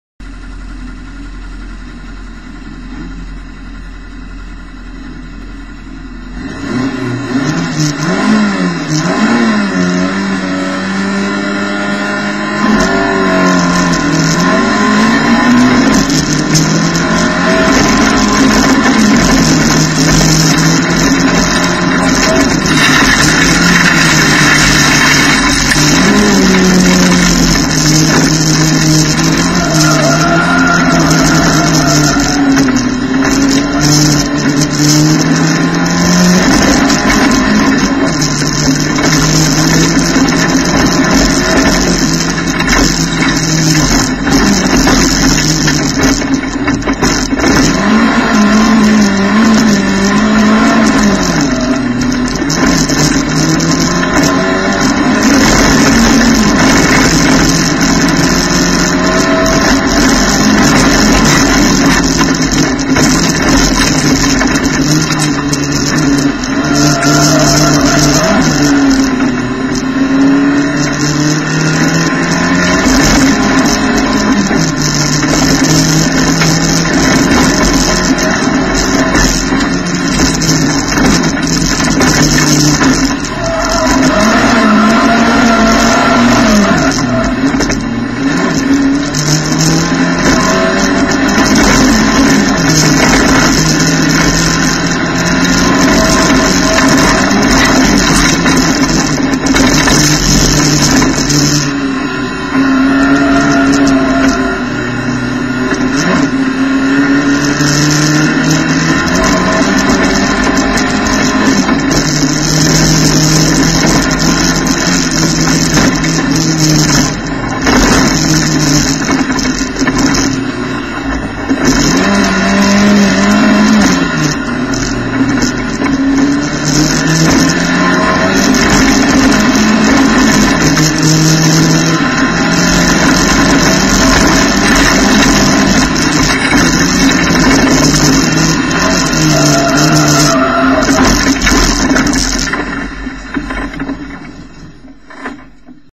jokamiehen kiekaus 2013 yleinen b finaali
sijoituslähtö jäi kuvaamatta mut siinä sijoitus toinen ja nousu b finaaliin. B finaalissa oli jo moottori niin kuuma ettei vetäny kunnolla enää ja viimisen kierroksen ohitusyritys päättyy puskaan :D